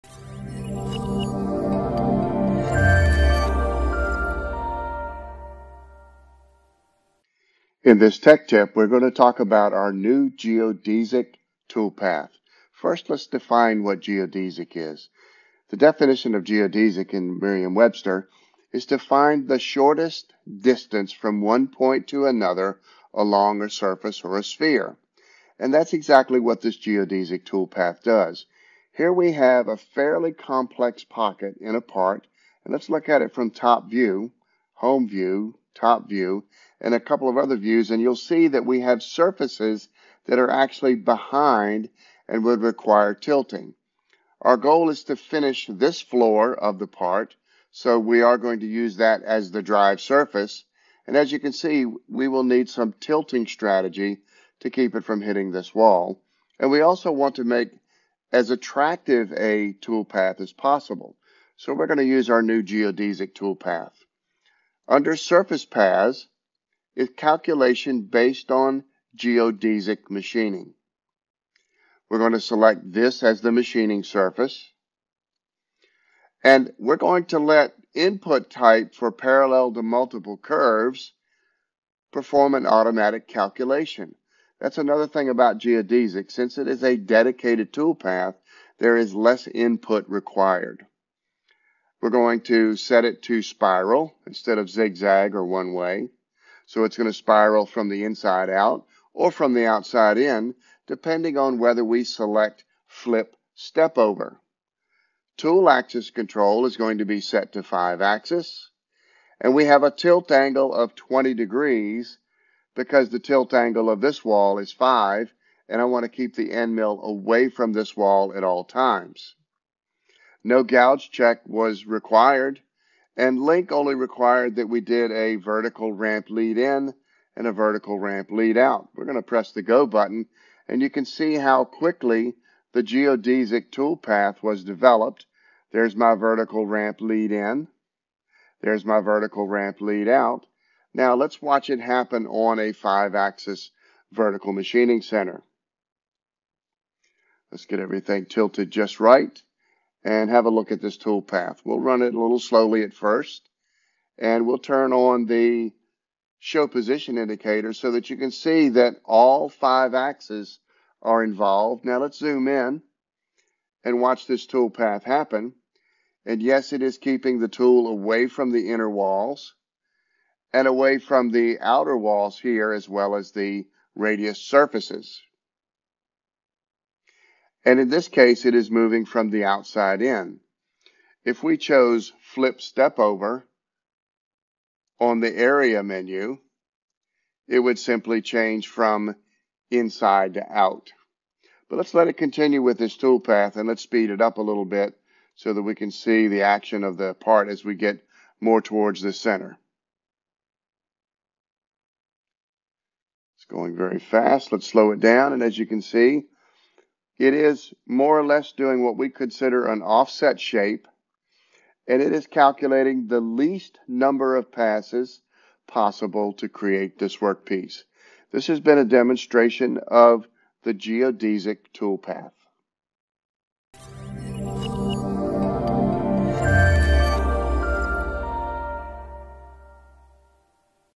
GibbsCAM Tech Tip: Using the Geodesic Toolpath
In this tech tip learn how GibbsCAM’s geodesic toolpath creates the most efficient 5-axis machining strategy.

With less input required and using the least amount of passes possible, this feature can help to cut programming time and increase efficiency.